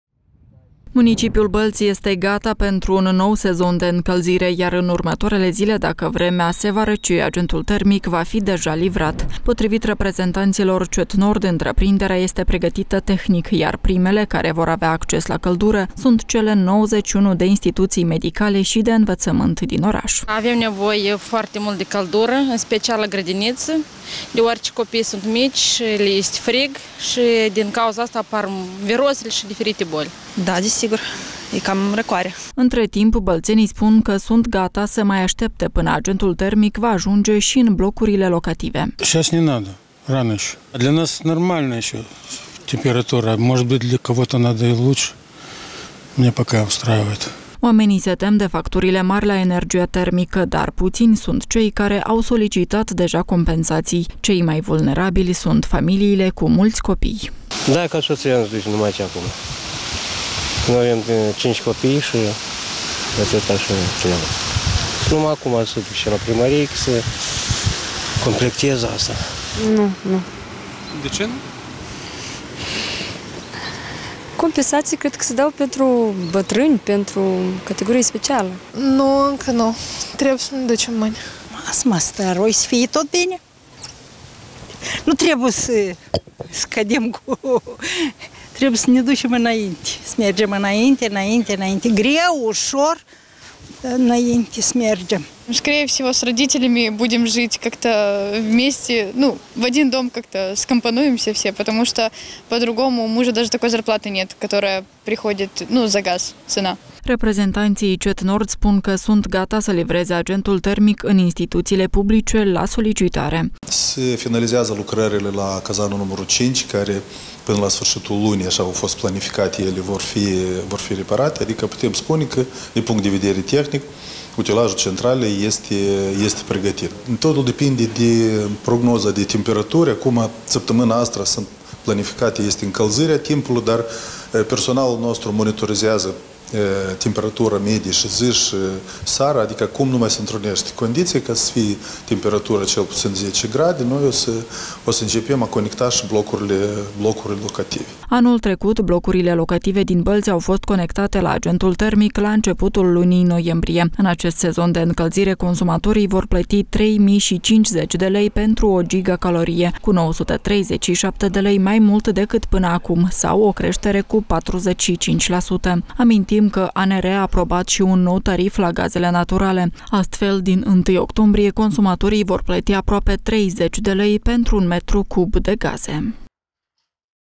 Municipiul Bălți, pregătit pentru sezonul de încălzire, dar localnicii sunt gata să mai aștepte
Municipiul Bălți este gata pentru un nou sezon de încălzire, iar în următoarele zile, dacă vremea se va răci, agentul termic va fi deja livrat. Potrivit reprezentanților „CET-Nord”, întreprinderea este pregătită tehnic, iar primele care vor avea acces la căldură sunt cele 91 de instituțiile medicale și de învățământ din oraș.